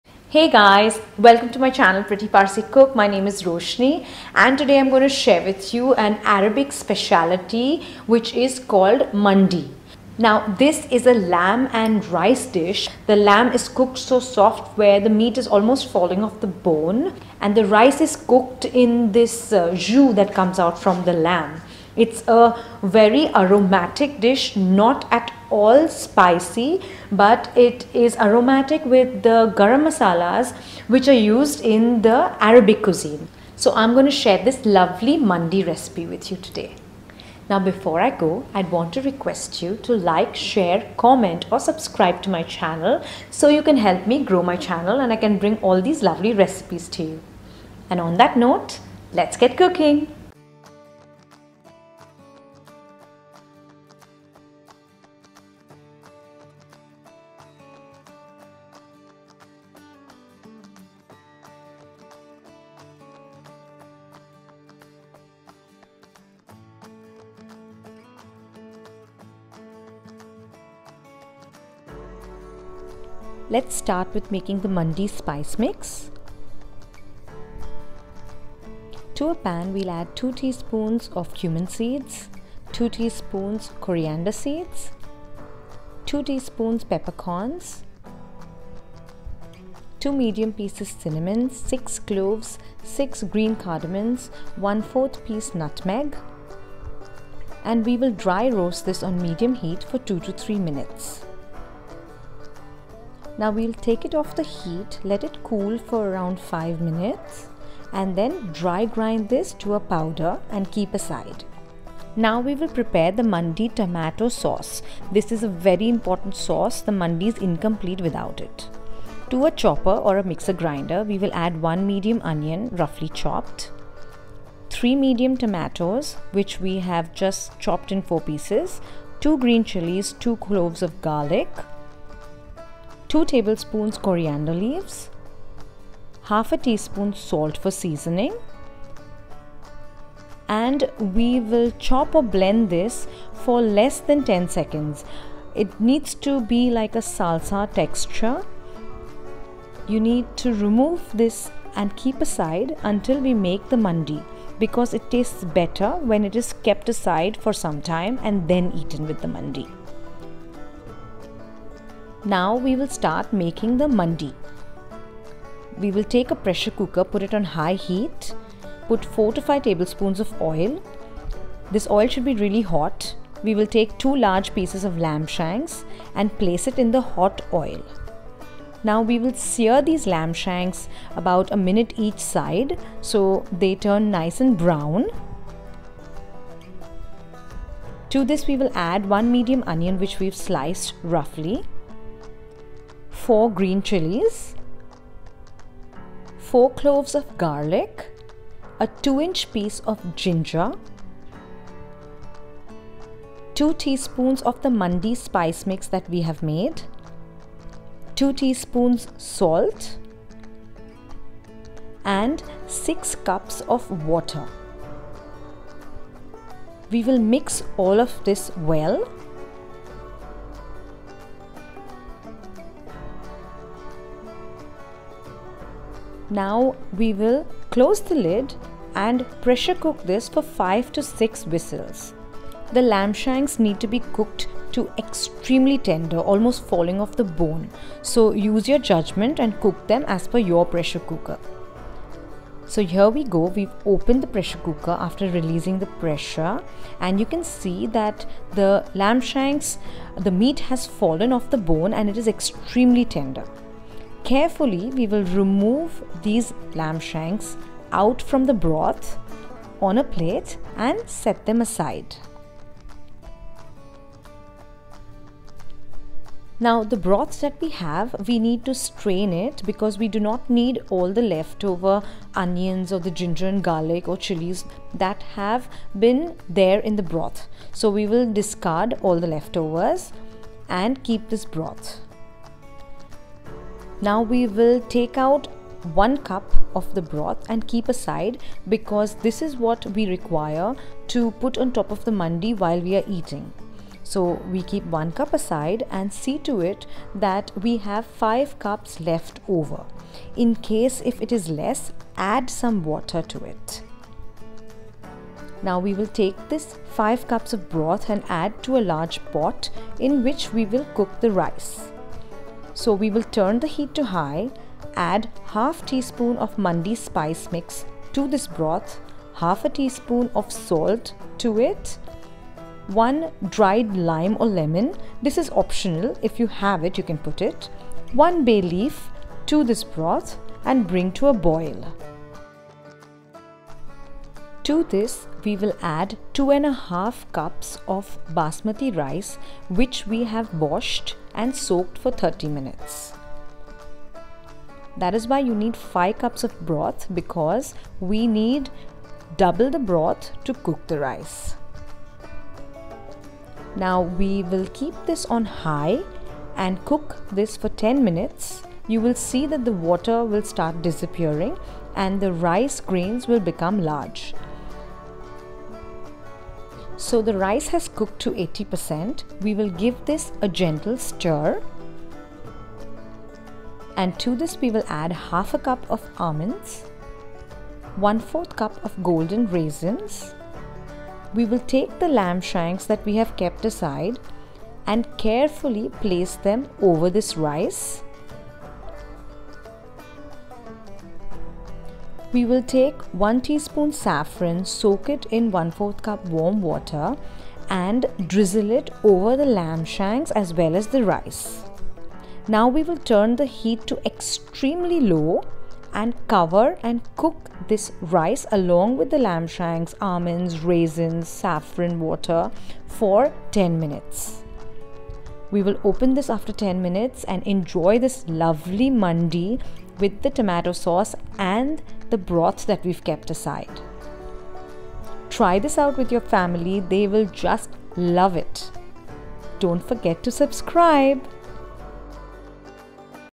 EASY Lamb Mandi recipe | The tastiest Lamb and Rice dish you will ever eat
This is a super easy and delicious Lamb Mandi recipe.
You just have to make this

Ingredients
Ingredients for Mandi Spice Mix
2 teaspoons Cumin seeds
2 teaspoons coriander seeds
2 teaspoons Pepper Corns
6 cloves
1/4 pc Nutmeg
6 Green Cardamons
2 medium pcs Cinnamon sticks

Ingredients for Mandi
2 large pcs Lamb Shanks
4 tablespoons oil
1medium onion
4 green chillies
4 garlic cloves
2 inch pc Ginger
2 teaspoons salt
2 teaspoons Mandi Spice mix
2 and 1/2 cups Basmati RIce
1/2 teaspoon salt
1 Bay leaf
1 dried Lime optional
1/2 teaspoon Mandi Spice
1 teaspoon Saffron
1/2 cup Almonds
1/2 cup Golden Rasins

Ingredients For Tomato Sauce
1 medium onion
3 medium tomatoes
2 garlic cloves
2 green chillies
2 tablespoons Coriander leaves
1/2 teaspoon salt

Method
Mandi Spice mix
Dry roast 2 teaspoons Cumin seeds, 2 teaspoons coriander seeds,
2 teaspoons Pepper Corns, 6 cloves, , 6 Green Cardamons,1/4 pc Nutmeg,
2 medium pcs Cinnamon sticks on medium heat  for 2 to 3 minutes
Remove the Pan and set aside until it cools, about 5 minutes
Dry grind this to powder and keep aside

Mandi Tomato sauce
Take a blender or a chopper and add to this 1 medium onion, 3 medium tomatoes, 2 garlic cloves, 2 green chillies, 2 tablespoons Coriander leaves,
1/2 teaspoon salt and blend or roughly chop for 10 seconds or less
You should get a salsa like consistency
Set this aside until we prepare the Mandi

Mandi prep
Place a pressure cooker on high heat and add to this 4 tablespoons oil
When the oil is hot add the meat (2 large pieces of Lamb Shanks)
about 1 kg
Sear the meat for 1 minute each side, until it is nice and brown
Add to this 1medium onion, 4 green chillies, 4 garlic cloves , 2 inch pc Ginger, 2 teaspoons Mandi Spice mix, 2 teaspoons salt and 6 cups water
Mix everything well and pressure cook for 5 to 6 whistles
The meat should be extremely tender - falling off the bone
Open the cooker after it has cooled and all the pressure is released
Check if the meat is nice and tenter
Remove the Shanks carefully in a plate and keep aside
There will be a lot of meat broth left over
Strain the broth and discard all the onion, chilli etc which is left over
Remove 1 cup from the broth and keep aside
This will be used to have as a side gravy for the Mandi
Now with the left over broth, you should have 5 cups remaining
Incase it is less then add a cup of water to make it exact 5 cups
Take this broth and add it to a large pot in which you will be cooking the rice for the Mandi
Add to this broth 1/2 teaspoon Mandi spice mix, 1/2 teaspoon salt,
1 bayleaf, 1 dry lime or lemon ( optional) and bring to a boil turning the heat to high
Add 500 gms Basmati rice ( washed and soaked for 30 mins) to this
Boil this for 10 minutes or until the rice is 80% cooked
Give everything a stir and add 1/2 cup Almonds, 1/4 cup Golden Rasins over the rice
Place the cooked lamb shanks over this
Finally take 1 teaspoon saffron and add 1/4 cup warm water to it, mix it and drizzle over the rice and lamb shanks
Now turn the heat to low, cover and cook this for another 10 minutes
Open and enjoy this aromatic Lamb Mandi with the tomato sauce and the Meat broth